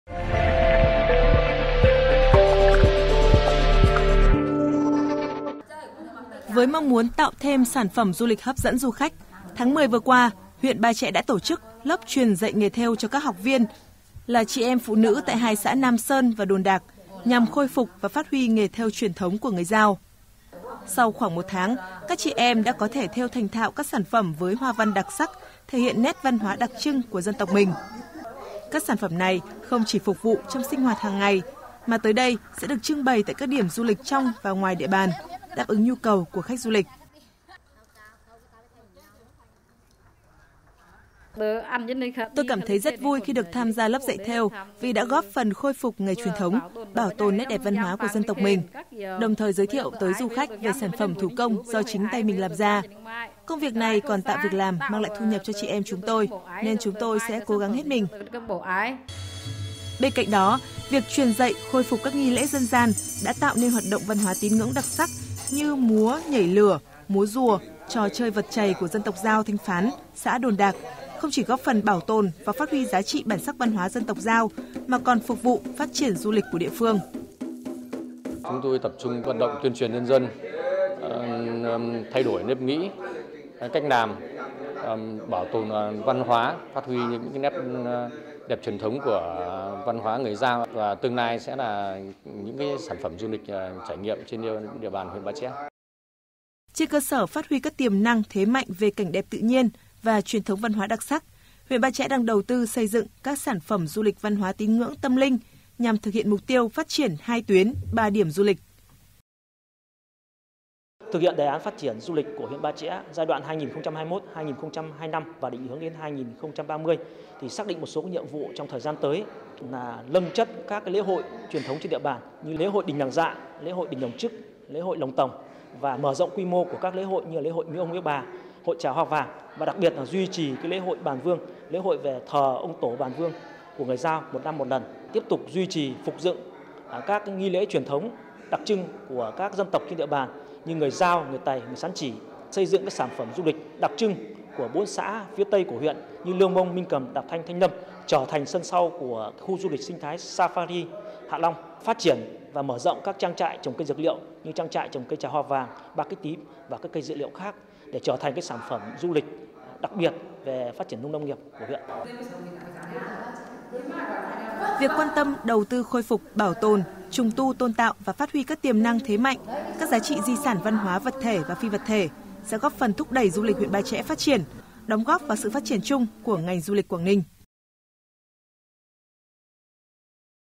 Ba Chẽ hướng tới phát triển du lịch | QTV
Những năm gần đây, Ba Chẽ tập trung triển khai nhiều giải pháp, nhằm bảo tồn văn hóa và phát triển tiềm năng thế mạnh của địa phương, thúc đẩy phát triển du lịch, góp phần nâng cao đời sống người dân.